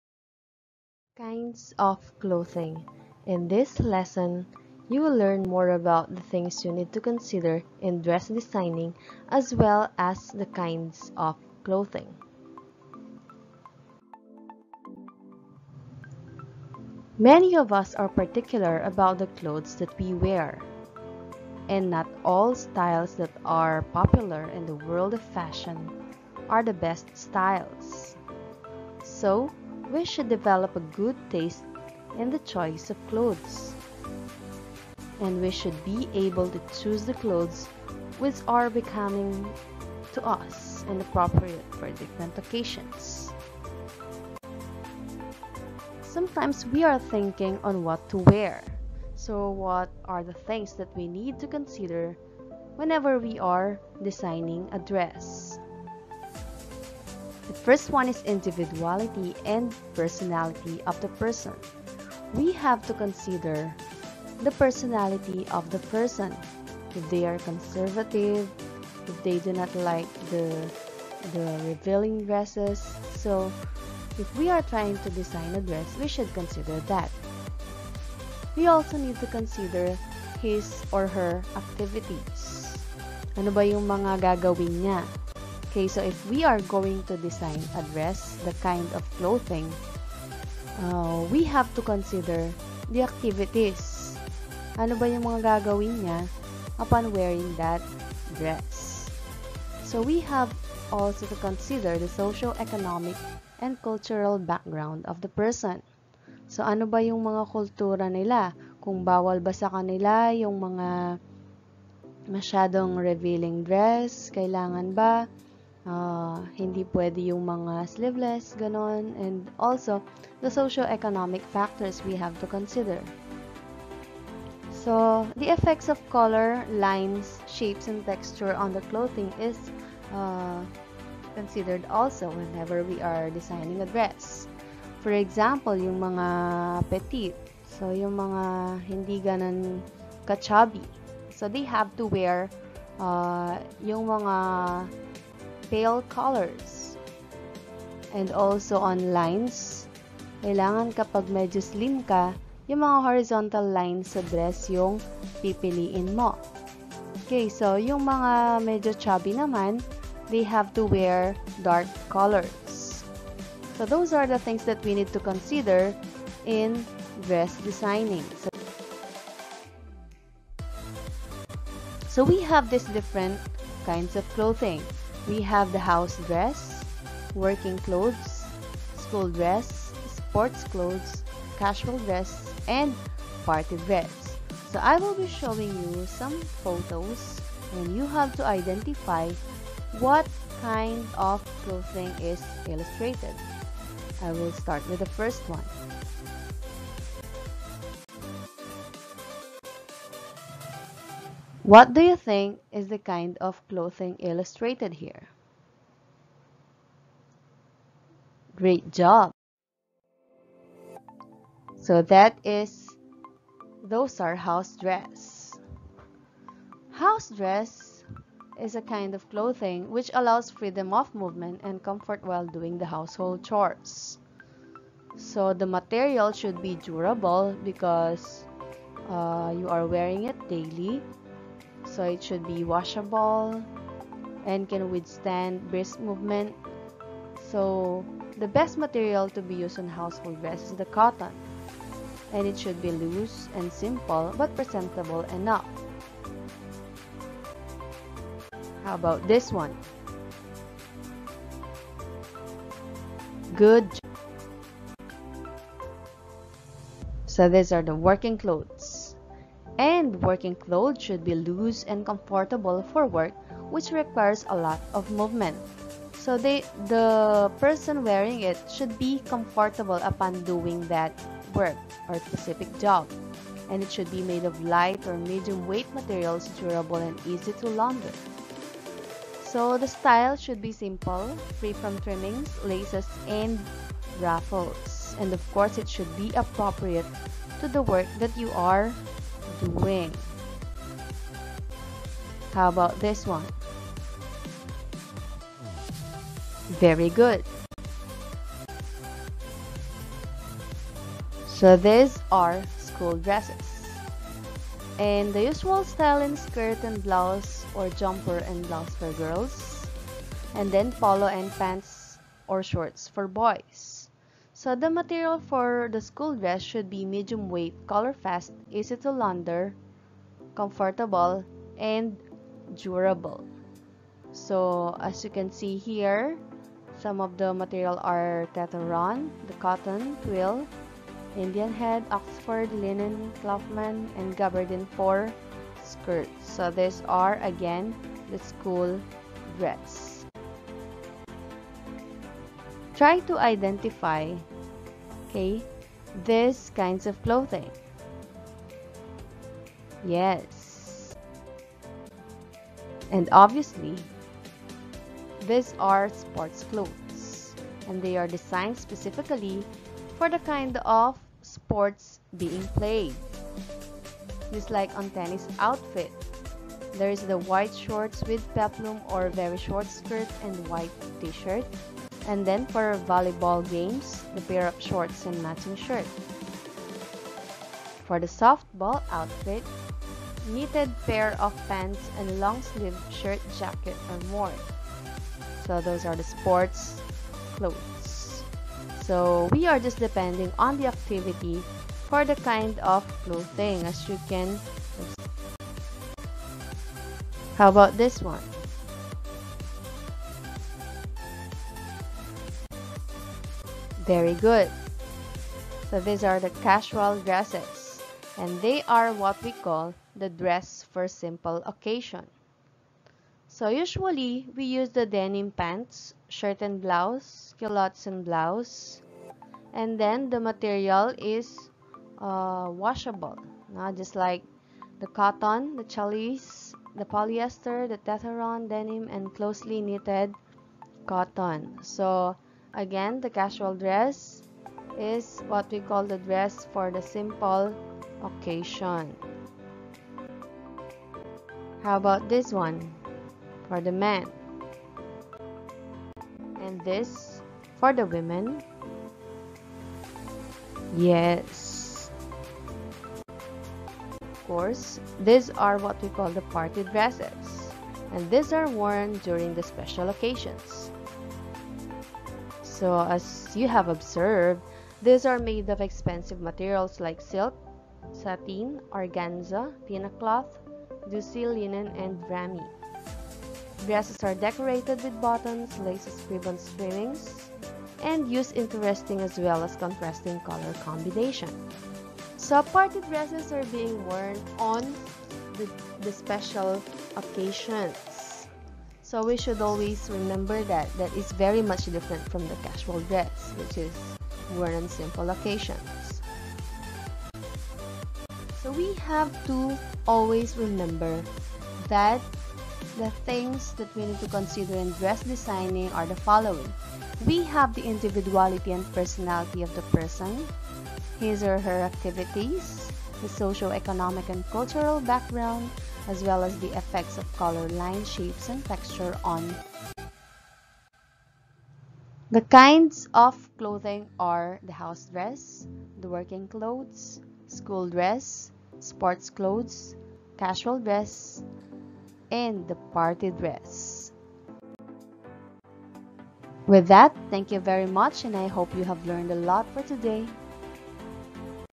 Kinds Of Clothing
This video discusses the considerations in Dress designing and Kinds of Clothing.